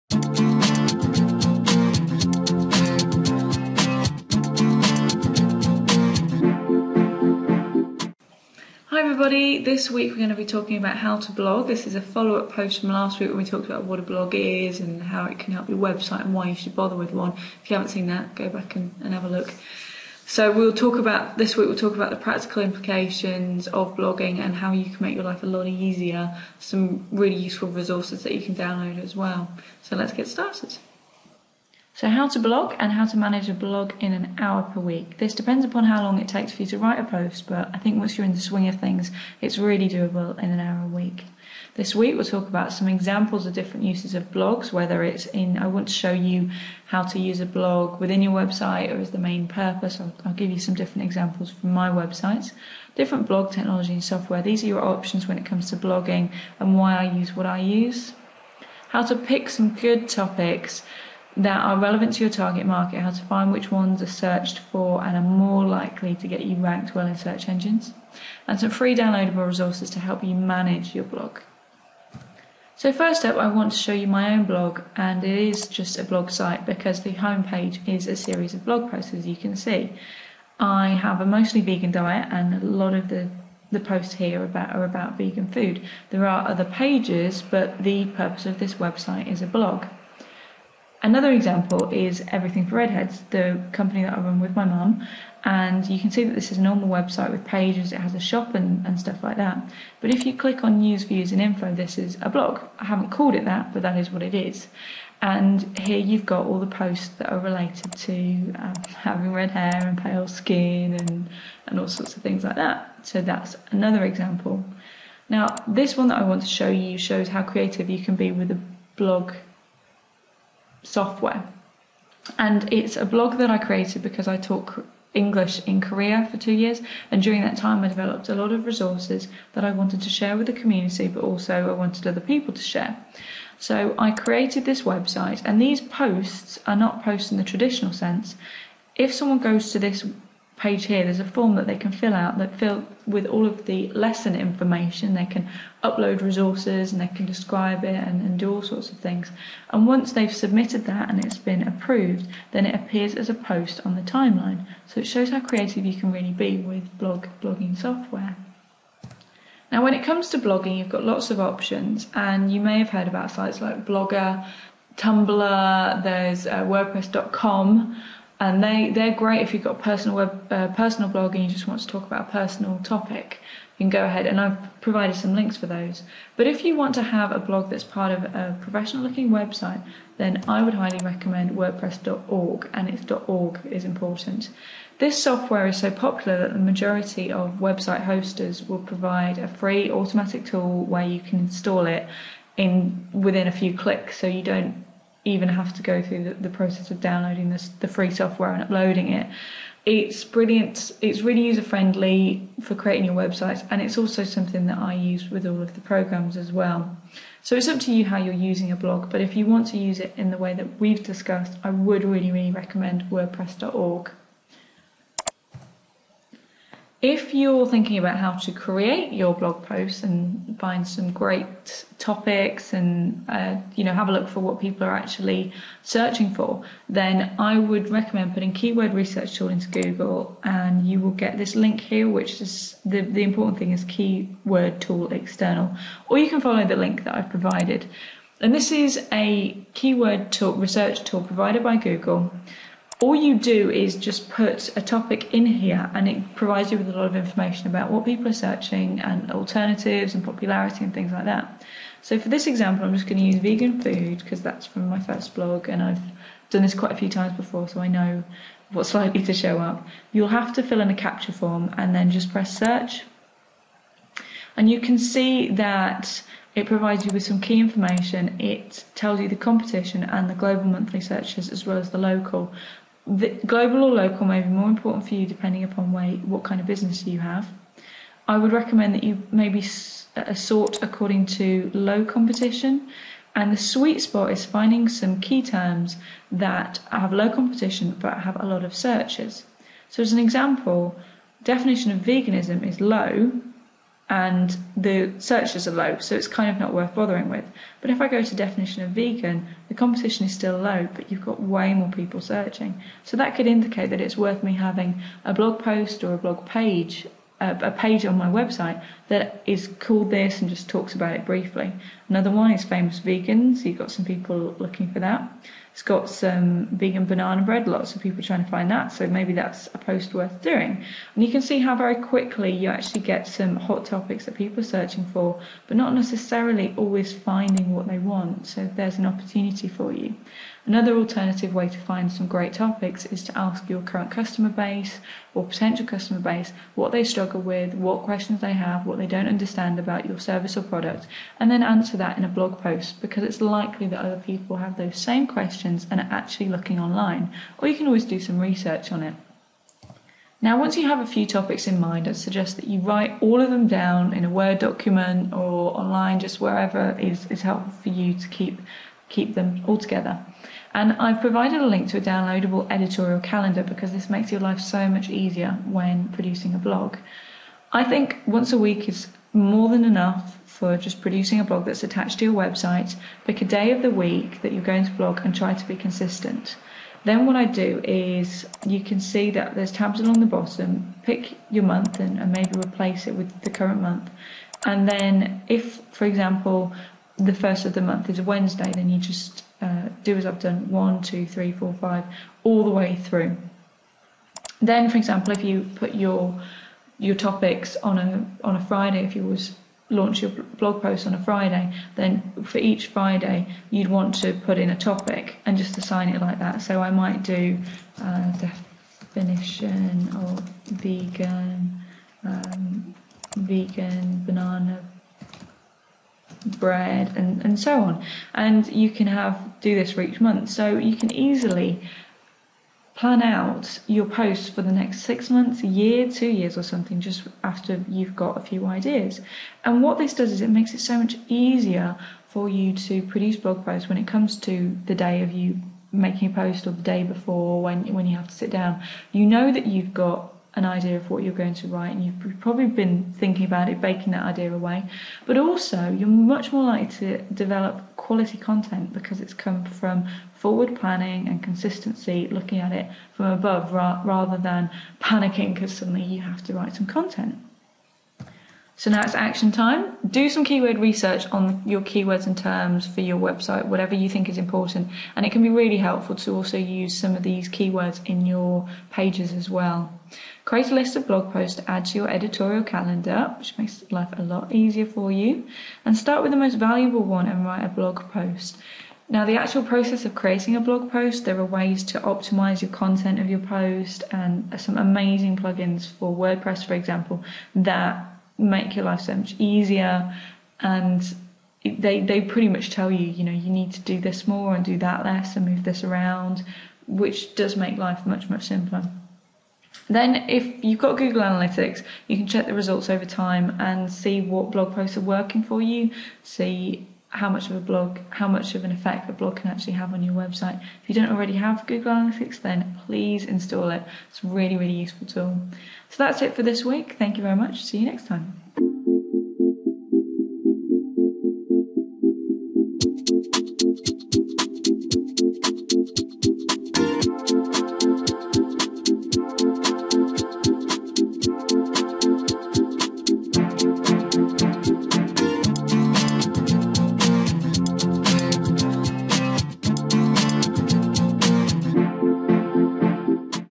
How to blog?
Last week we talked about what a blog is, why you should blog and why it works for your business website - but you may still be wondering -How to Blog?-. How do you practically put it into place...

If you're only interested in blogging for a personal interest, then free platforms like Blogger, WordPress.com, Tumblr and more. However, if you want a blog that's integrated with your business website, I have found the software from WordPress.org (NOT .com) to be perfect! Its free software that I use on all of my self hosted websites, and its different from the rest because you really can produce a professional website that doesn't look like its part of someone else's site. Its also easy to set up a blog full integrated with the whole of the rest of the site.

But that doesn't really tell you HOW to blog does it? Well there are some strategies that make life a little easier when starting to blog, helps you find content to write on.

Please go here for all the downloads promised in the video